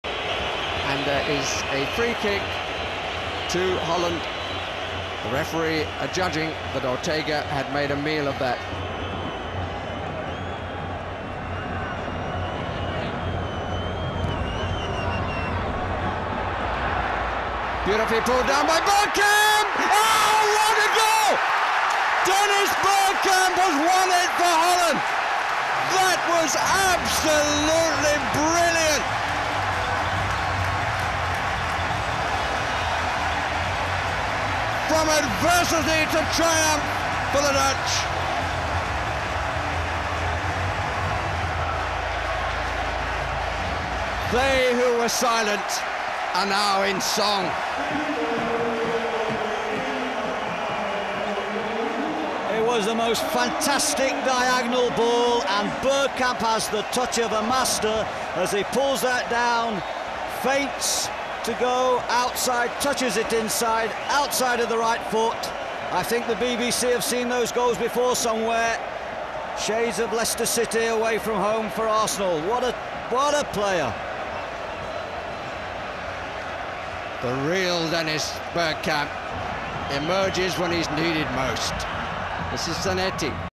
Dennis Bergkamp goal WK 1998 (HQ 4:3) with Barry Davies BBC commentary
Dennis Bergkamp Amazing Goal vs Argentina.
"They who were silent are now in song"
BBC recording in original aspect ratio!
Source: Sony Beta HiFi SLC-100